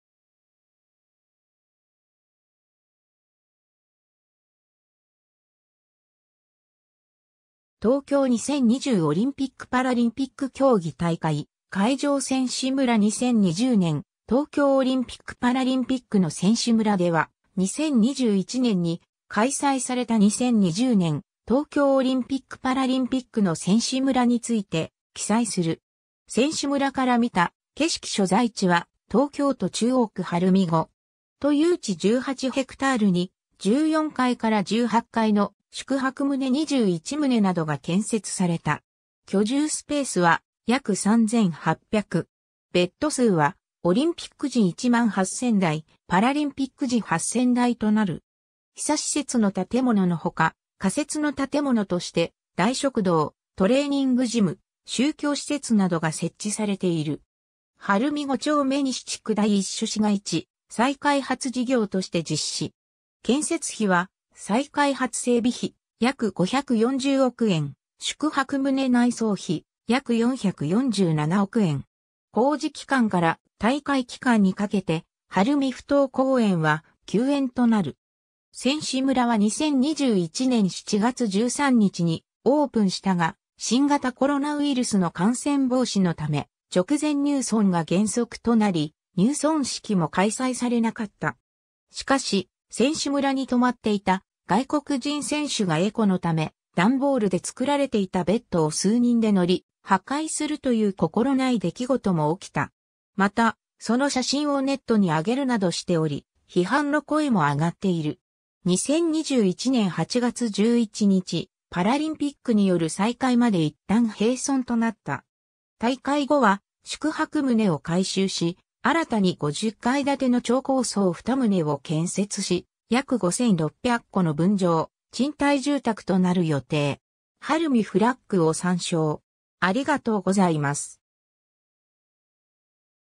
2020年東京オリンピックの選手村
東京2020オリンピック・パラリンピック競技大会 」 会場 」 選手村 2020年東京オリンピック・パラリンピックの選手村（2020ねんとうきょうオリンピック・パラリンピックのせんしゅむら）では、2021年に開催された2020年東京オリンピック・パラリンピックの選手村について記載する。
 選手村から見た景色 所在地は東京都中央区晴海5。
都有地18ヘクタールに、14階から18階の宿泊棟21棟などが建設された。
居住スペースは約3800。
ベッド数はオリンピック時1万8000台、パラリンピック時8000台となる。
 恒設の建物のほか、仮設の建物として、大食堂、トレーニングジム、宗教施設などが設置されている。
 晴海五丁目西地区第一種市街地再開発事業として実施。
建設費は再開発整備費約540億円、宿泊棟内装費約447億円。
 工事期間から大会期間にかけて晴海ふ頭公園は休園となる。
 選手村は2021年7月13日にオープンしたが、新型コロナウイルスの感染防止のため直前入村が原則となり、入村式（歓迎式典）も開催されなかった。
 しかし、選手村に泊まっていた外国人選手がエコのため、ダンボールで作られていたベッドを数人で乗り、破壊するという心無い出来事も起きた。
また、その写真をネットにあげるなどしており、批判の声も上がっている。
 2021年8月11日、パラリンピックによる再開まで一旦閉村となった。
 大会後は宿泊棟を改修し、新たに50階建ての超高層2棟を建設し、約5600戸の分譲・賃貸住宅となる予定。
HARUMI FLAGを参照。